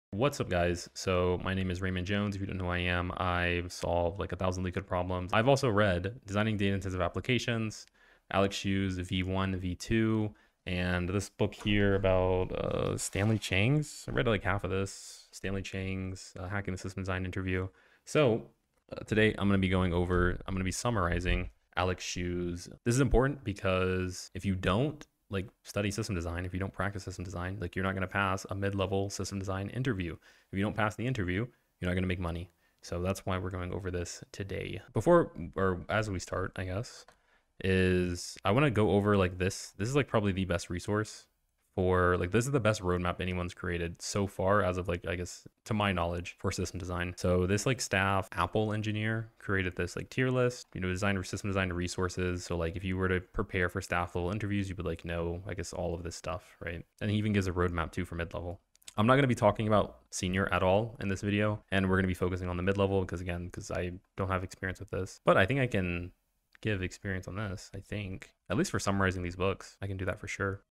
An Senior Apple Engineer Made One of the Best System Design Roadmaps
This is one of the best roadmaps I've seen for System Design